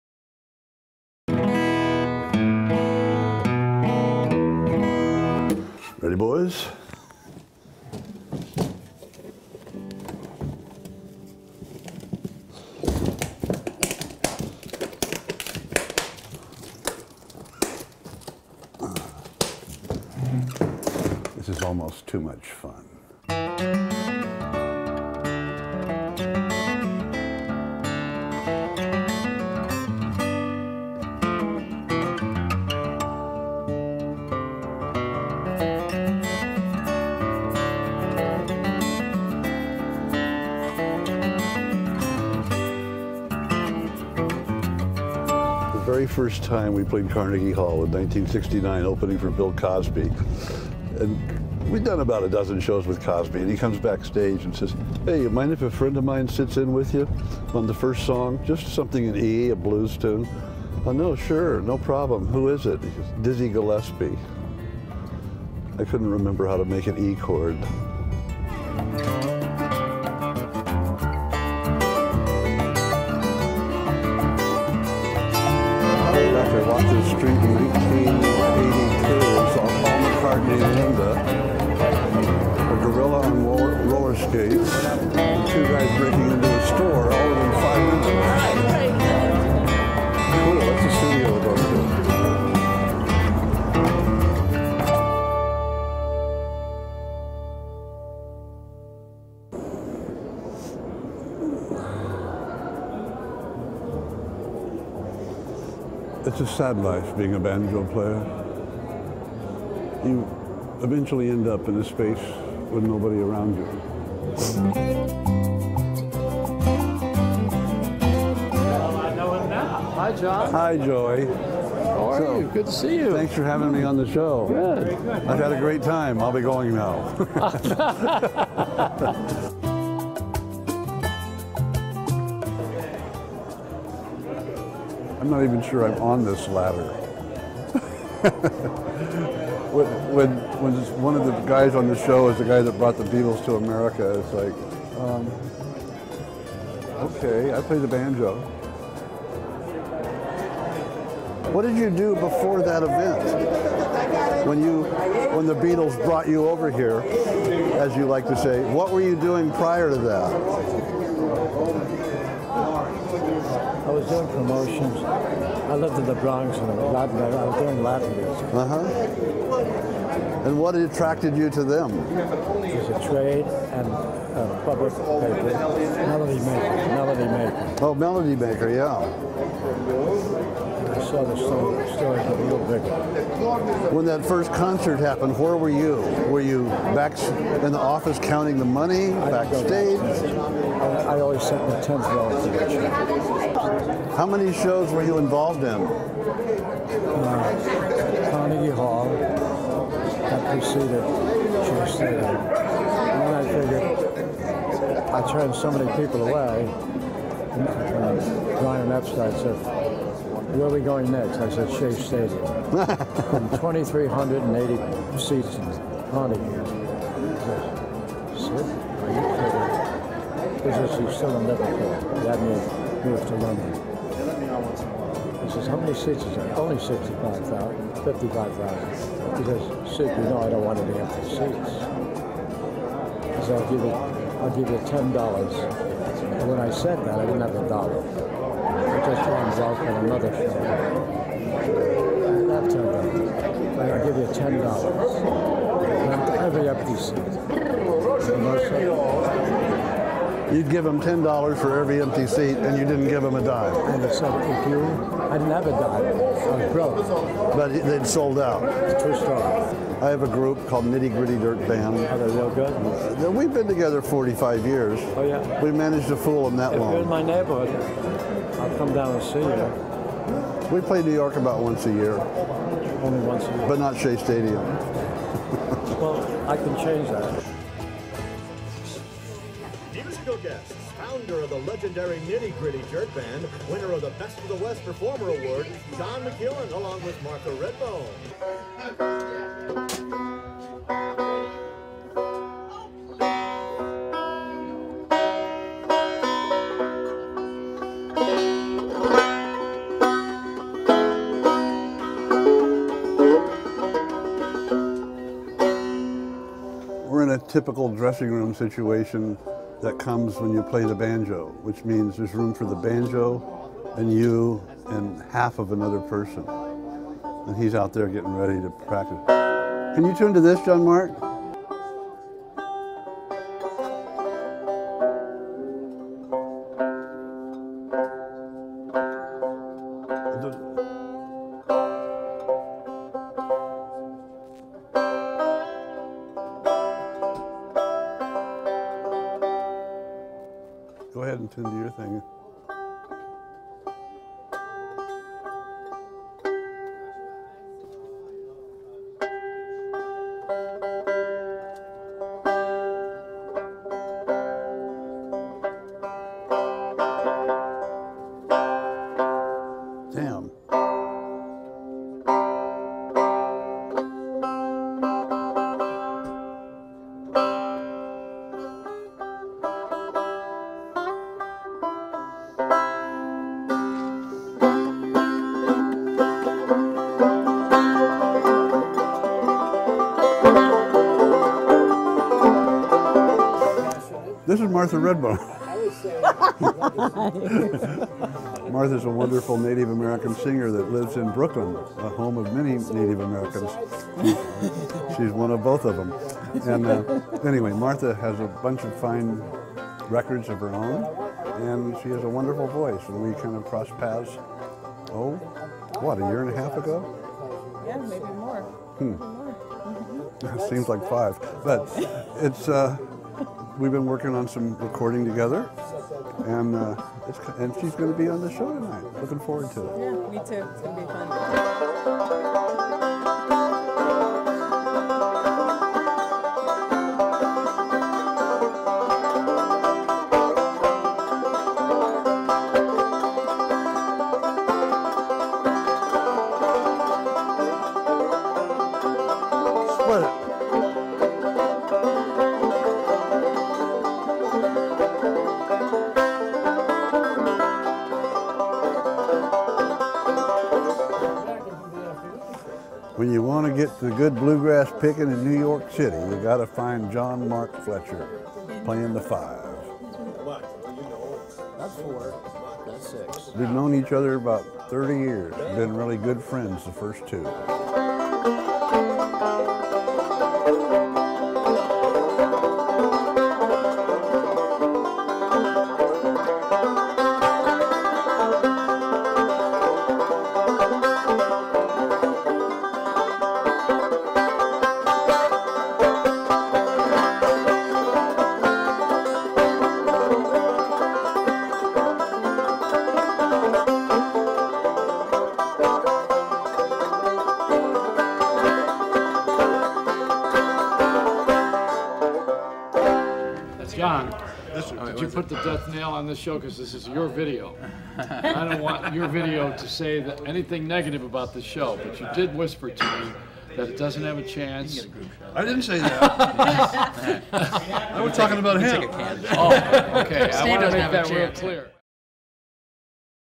A Night In NYC with John McEuen
One night while living in the Apple I went to Times Square to do a t.v. show with my new friend Joey Reynolds.. and met Sid Bernstein, the man who first brought the Beatles to America! and played the banjo, and brought on some guests, and and and.. well, check it out.